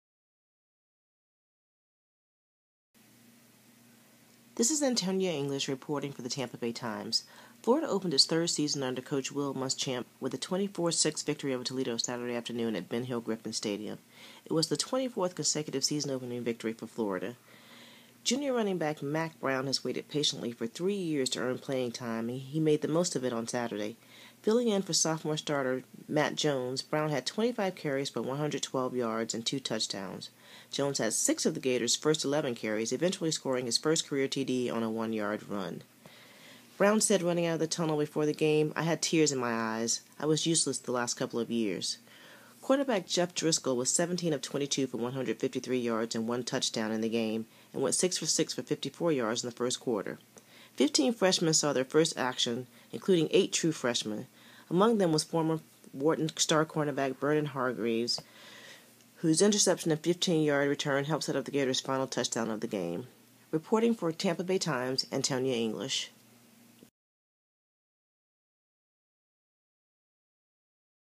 Tampa Bay Times Florida-Toledo recap
Tampa Bay Times reporter Antonya English recaps Florida's 24-6 win over Toledo